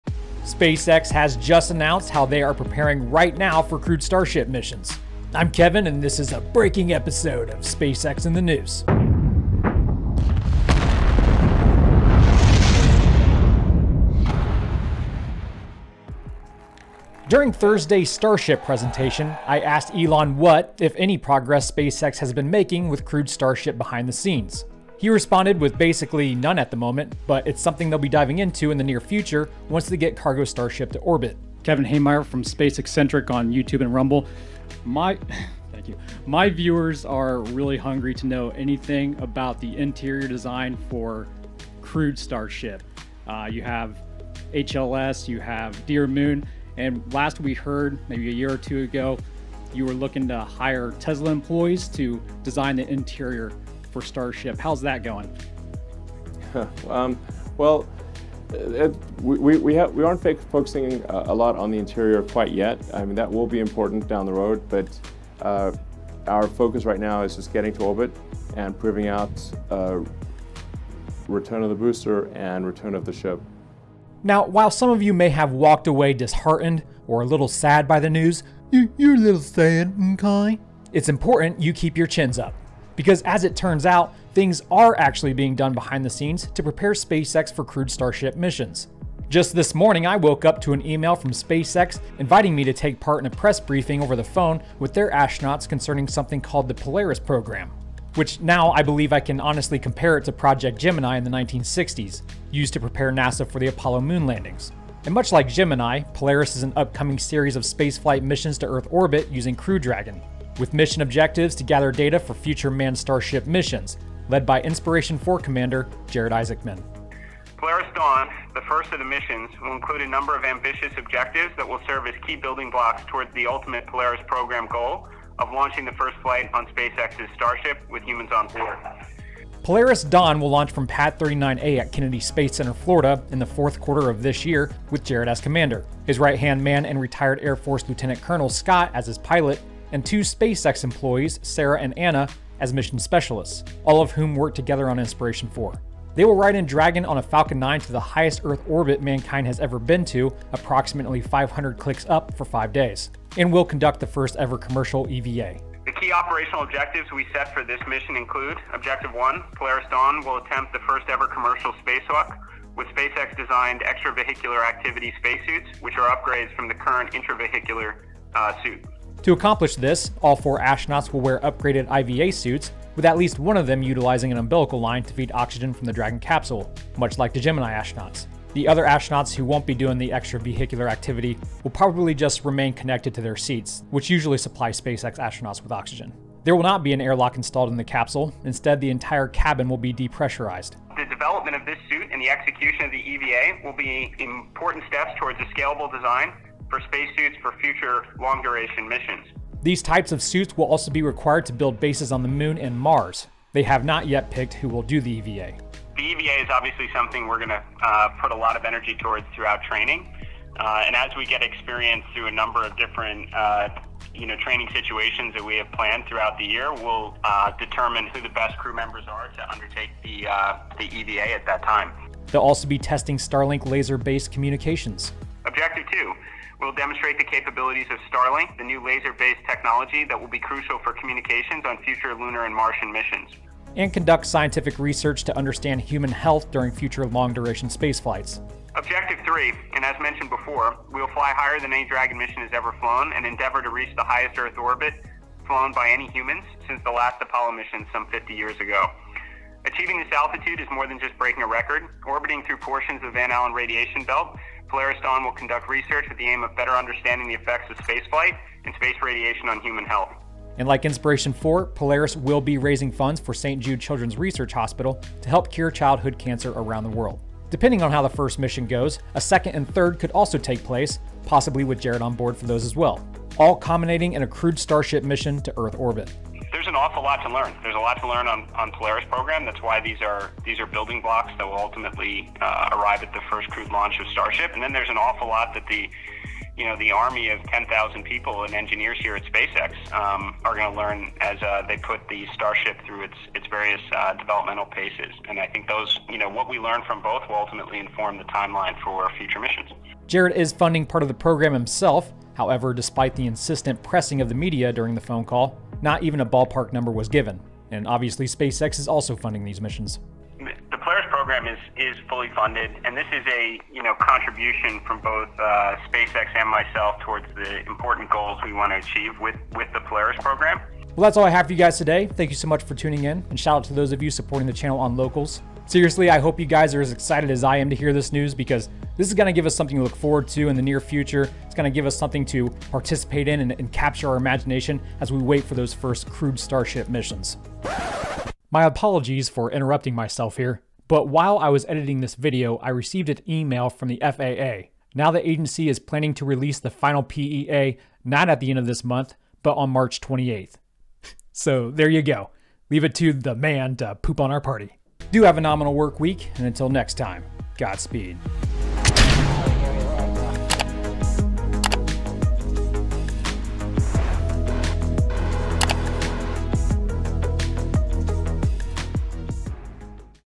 SpaceX Announces Polaris Program to Prepare for Crewed Starship Missions, FAA Further Delays PEA
Everything you need to know about SpaceX's new Polaris Program. Plus the FAA once again delays the release of their PEA. So it will be at least an additional month until Starship Super Heavy can launch to orbit. Ep 298.

THUMBNAIL IMAGE: Polaris Program